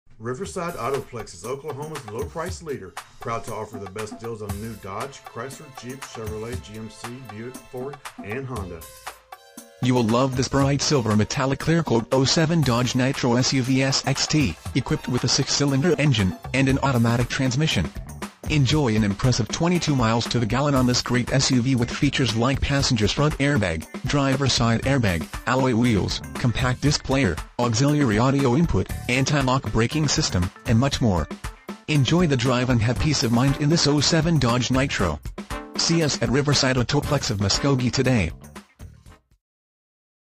2007 Dodge Nitro Riverside Autoplex of Muskogee Muskogee, OK 74401
Dodge Nitro Check out this Bright Silver Metallic Clearcoat Dodge Nitro SUV, equipped with a 6 Cylinder engine and an automatic transmission. Enjoy an impressive 22 miles to the gallon on this great SUV with features like Passenger's Front Airbag, Driver Side Air Bag, Alloy Wheels, Compact Disc Player, Auxiliary Audio Input, Anti-Lock Braking System, Tinted Glass, Auxiliary Transmission Oil Cooler, Tire Pressure Monitoring System, Cruise Control, Rear Bench Seat, and much more. Enjoy the drive and have peace of mind in this Dodge Nitro. See us at Riverside Autoplex of Muskogee today!
MILES: 140,849
VIN:1D8GT28K17W554051

1711 West Shawnee
Muskogee, Oklahoma 74401